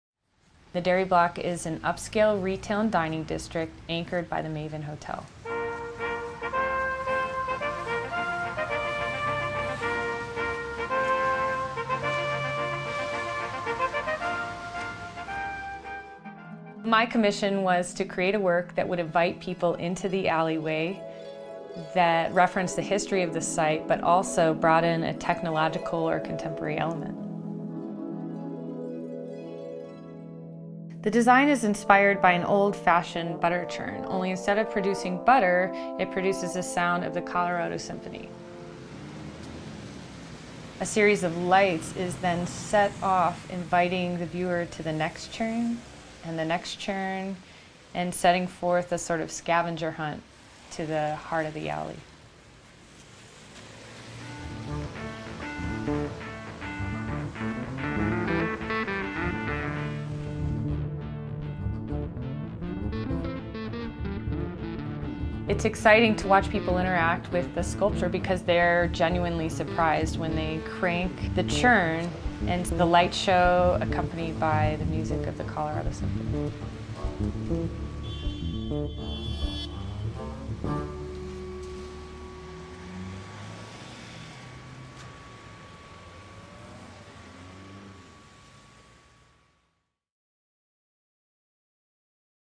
Musical Churns
Woven throughout the Dairy Block Alley at 1800 Wazee Street in Denver, Colorado, visitors will experience an interactive sculptural installation. The design based in recognition of the site’s origination, Windsor Farms Dairy features seven bronze cast butter churns in which guests are invited to engage. The Musical Churns incorporate musical compositions synchronized together with choreographed LED performances to engage and guide viewers in their own magical exploration of The Alley. Perhaps the most spectacular component, the aural delight, was composed by Colorado Symphony Musician’s: Justin Bartels, Courtney Hershey Bress, William Hill, Ben Odhner, Kolio Plachkov, Nicholas Recuber, and John Sipher.